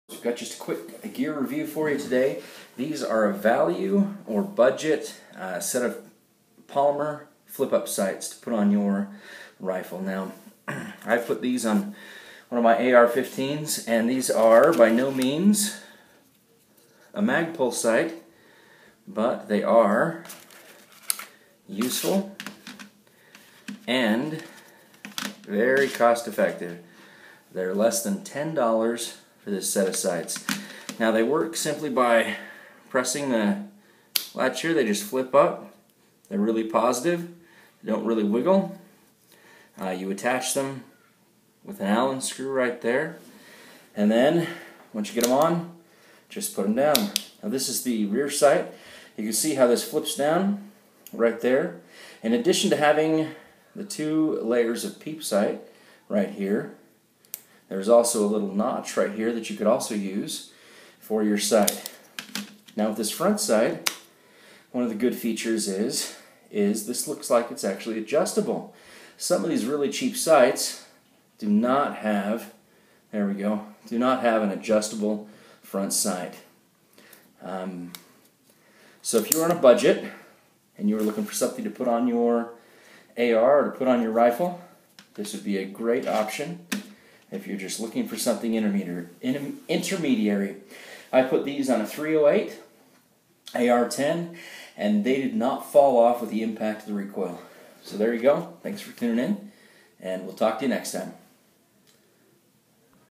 Cheap Flip Up Sights (Under 10 Bucks!)
Back Up Sights. BUIS. MSR Sights. AR-15 Sights. Can you actually pay less for a functional set of back up sights? Many tried and true brands cost 80 or 90 dollars or more. Is there an effective low budget way to get something that works while you save for something better? We test out some of the cheapest plastic flip up sights available to see if they will work under range conditions. We think you will be surprised with the results.

Update! see our updated video! These sights developed some problems and have some drawbacks. We can no longer recommend them!